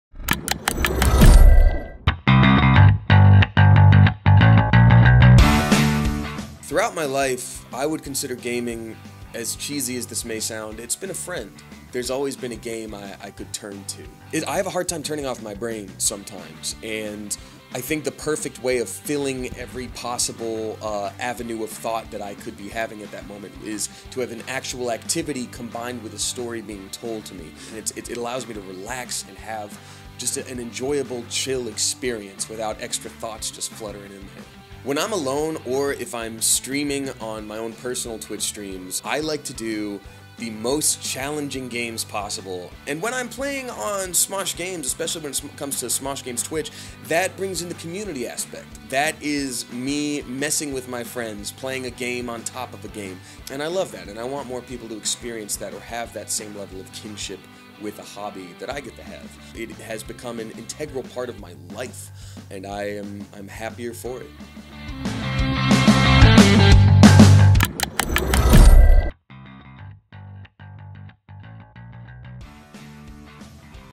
Damien Haas – SMOSH Games | Play to Progress | OMEN
Voice Actor, Gamer, content creator and SMOSH Games boi, Daminen Haas always had gaming as a friend and now gaming has become an integral part of his life.

About OMEN:
OMEN Official. We champion the journey of everyone who plays.